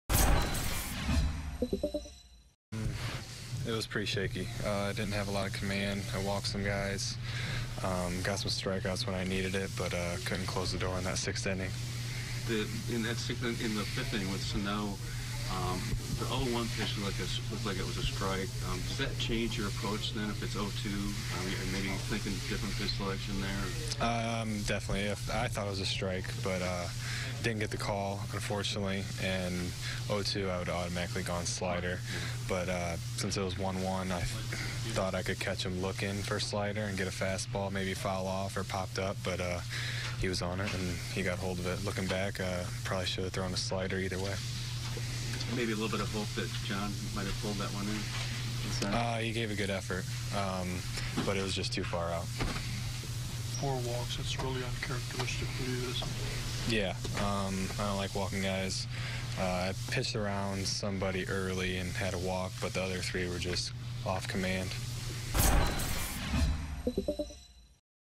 Jakob Junis says start against Twins was "pretty shaky"
Jakob Junis on issuing four walks against the Twins: "I don't like walking guys. I pitched around somebody early and had a walk, but the other three were just off-command."